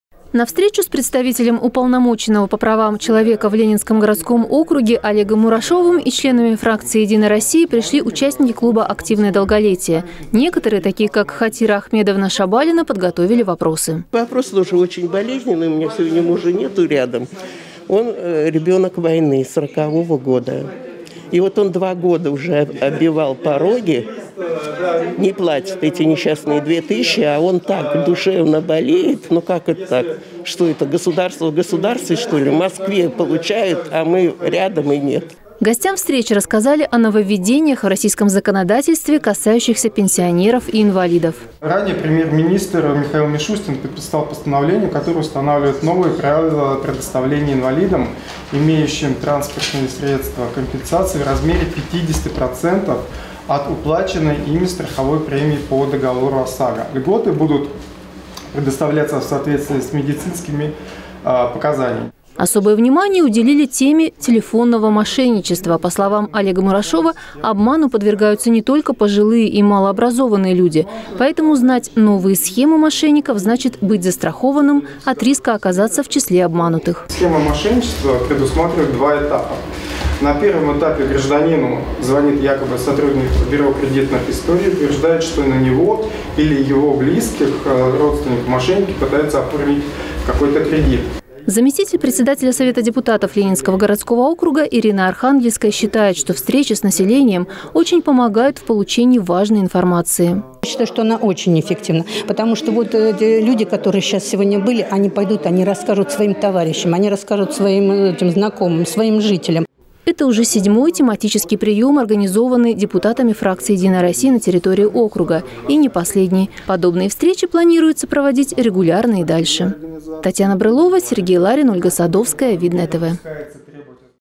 ПРИЁМ НАСЕЛЕНИЯ
В центре социального обслуживания «Вера» прошёл приём населения по вопросам социальной поддержки граждан пожилого возраста. Это уже 7-й тематическая встреча, организованная депутатами фракции «Единая Россия» на территории округа.